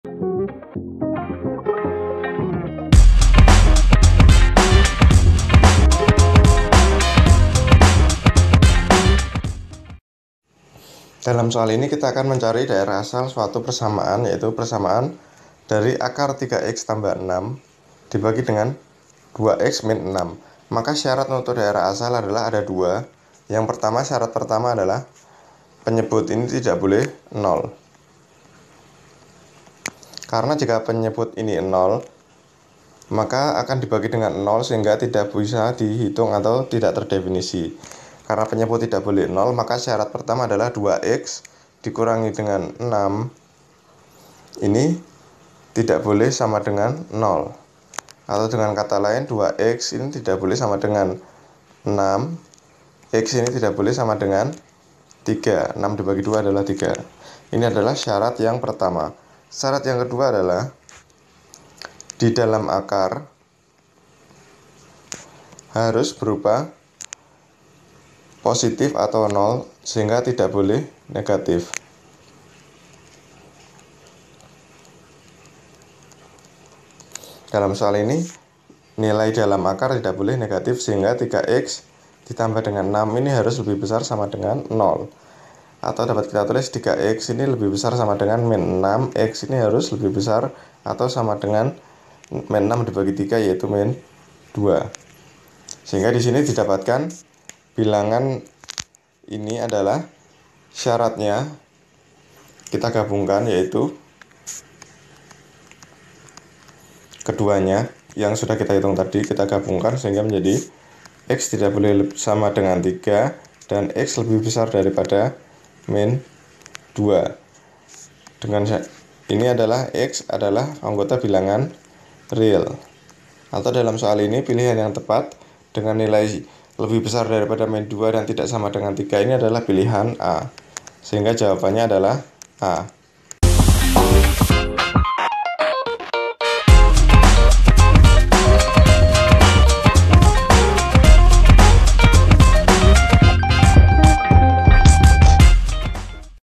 Daerah asal fungsi [tex]\\ f(x)= \frac{\sq... | Mandiri Matemat | Kelas 12 | Matematika
✅ Dalam video ini kita akan membahas: Daerah asal fungsi [tex]\\ f(x)= \frac{\sqrt{3x+6} }{2x-6}[/tex] adalah.
A. {x I x\geq -2, x\neq 3, x ∈ R}
B. {x I xless than -2, x\neq 3, x ∈ R}
C. {x I x \leq -2, x\neq 3, x ∈ R}
D. {x I x greater than -3, x\neq 3, x ∈ R}
E. {x I x \geq -2, x\neq 3, x ∈ R}
Tema:
Kelas: 12
Mata Pelajaran: Matematika
Bab:
Buku: Mandiri Matematika Untuk SMA/MA Kelas XII Kelompok Wajib

👉 Buku yang telah tersedia:
{PLAYLIST_LINK}
Di mana kamu dapat menemukan kami?